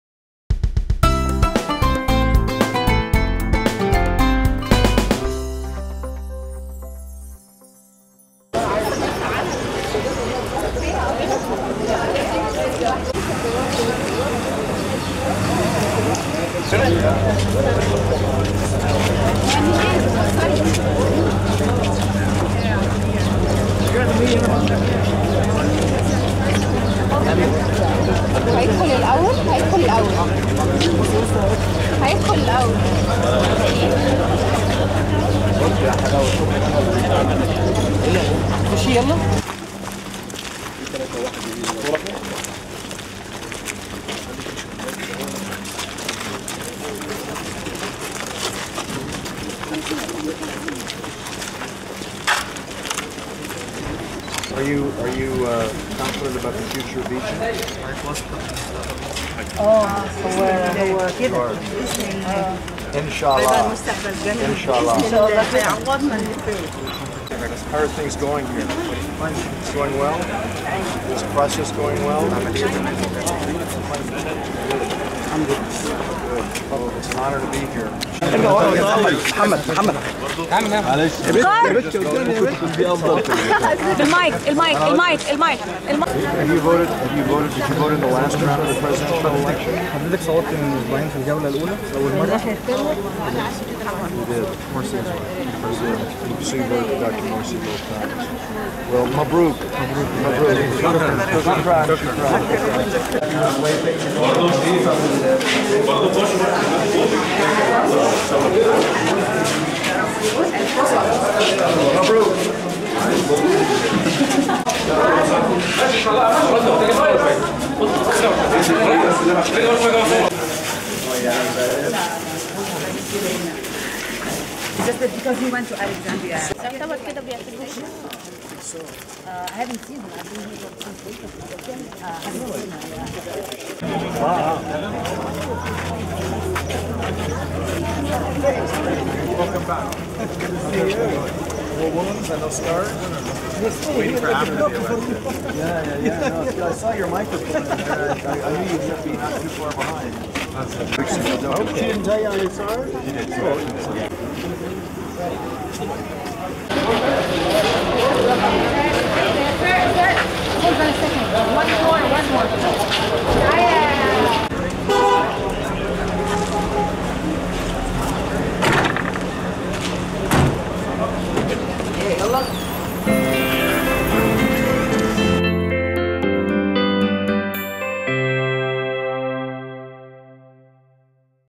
الوطن|براير عضو الكونجرس الأمريكي يراقب الانتخابات
جريدة الوطن

براير عضو الكونجرس الأمريكي يراقب الانتخابات